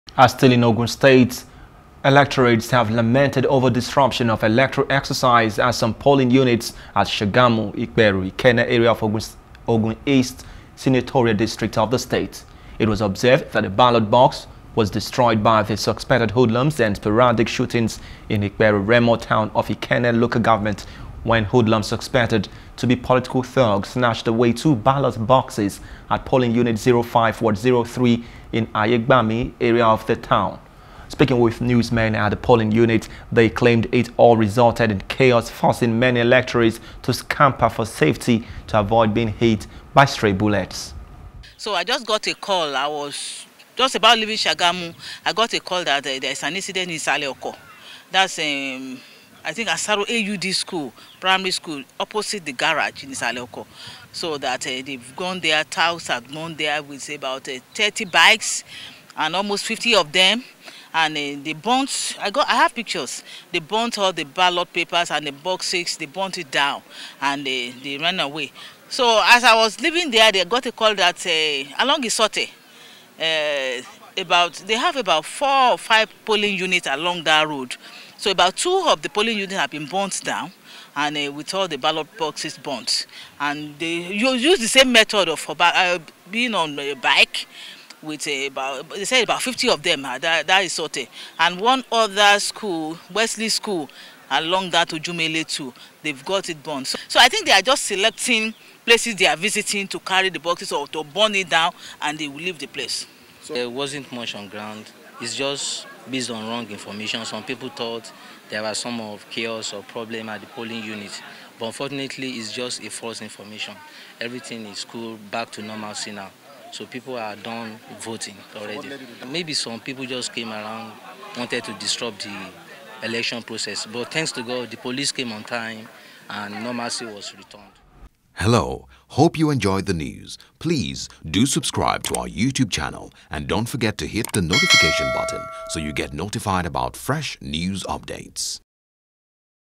Electorates Lament Over Disruption Of Ballot Box.
In Ogun State electorates have lamented over disruption of electoral exercise at some polling unit at Sagamu, iperu, ikenne area of Ogun East Senatorial district of the state.

it was observed that a ballot box was destroyed by the suspected hoodlums and sporadic shootings in Iperu Remo town of Ikenne Local Government when hoodlums suspected to be political thugs snatched away two ballot boxes at Polling Unit 05, Ward 03 in Ayegbami area of the tow